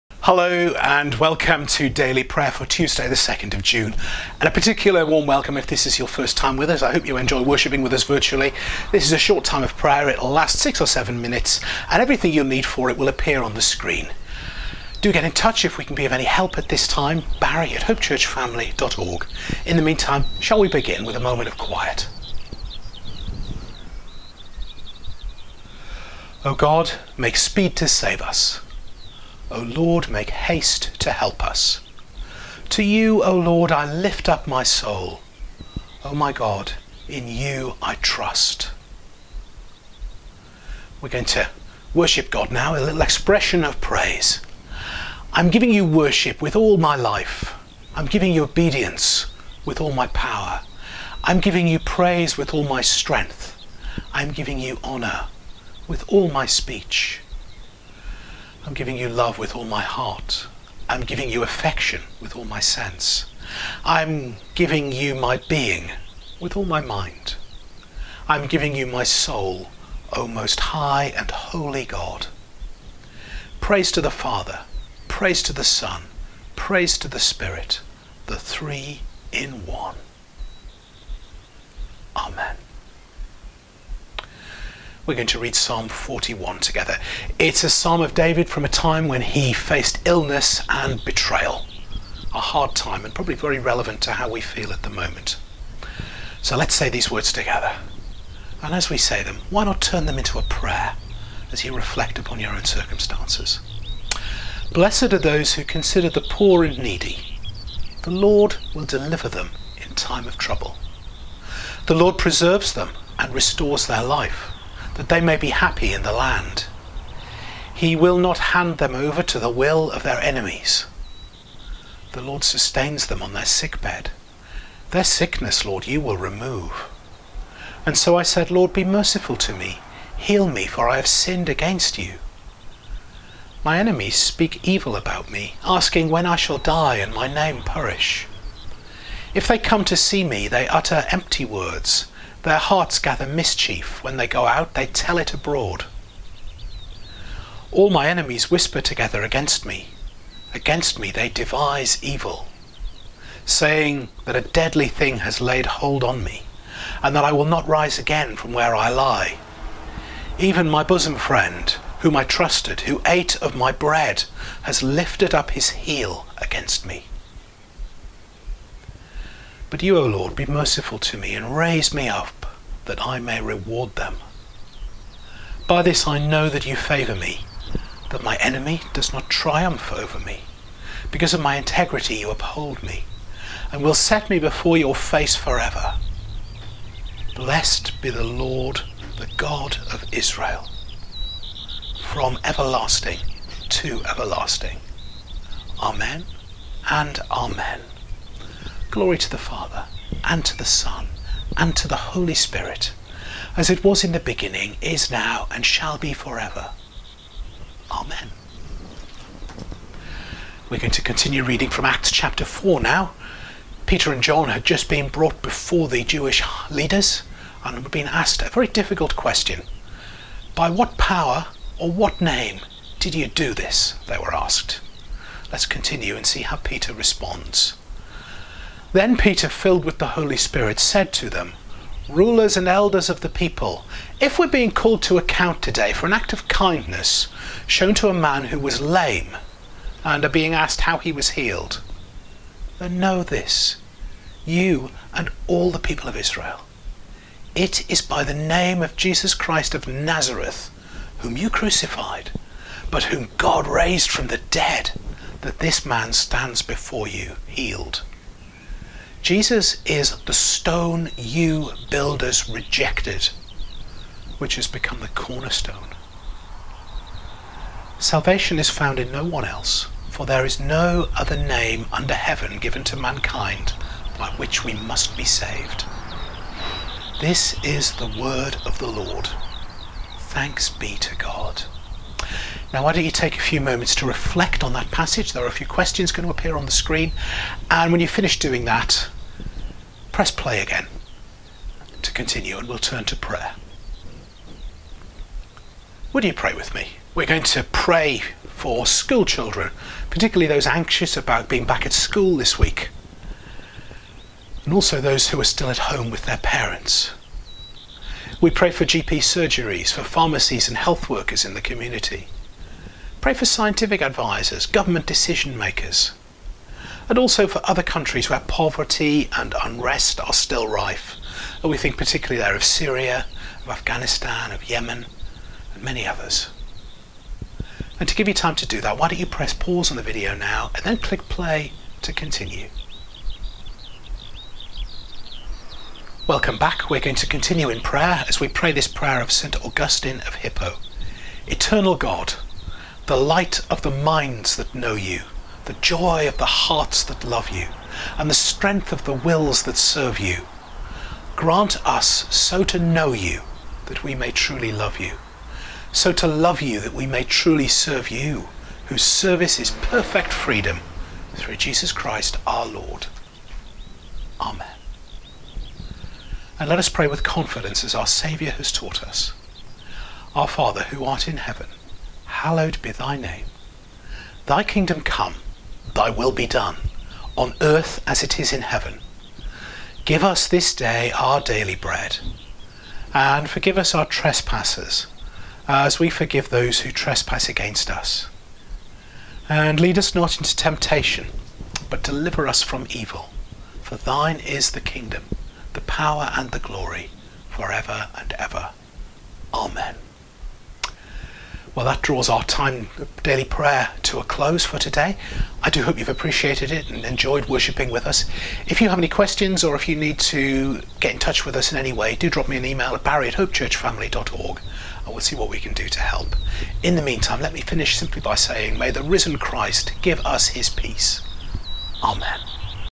Daily Prayer for 2 June 2020
Join Rev'd Barry Unwin for Daily Prayer.

HopeChurchFamily is a network of Church of England churches working in Upton on Severn and the surrounding villages.